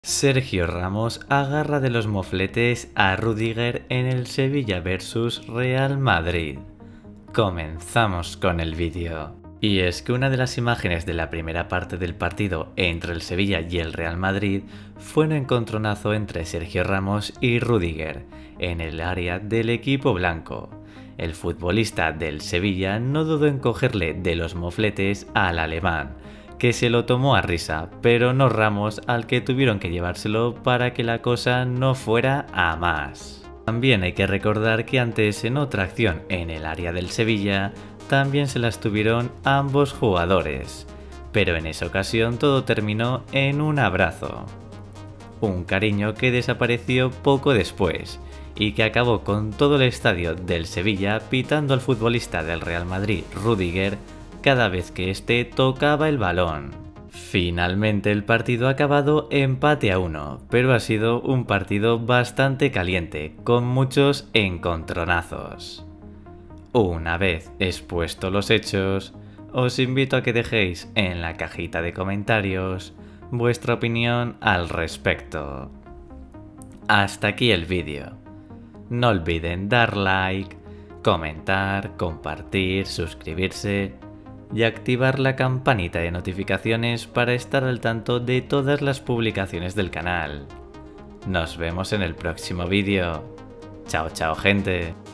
SERGIO RAMOS AGARRA DE LOS MOFLETES A RÜDIGER EN EL SEVILLA VS REAL MADRID
Sergio Ramos agarra de los mofletes a Antonio Rüdiger en el partido entre el Sevilla y el Real Madrid.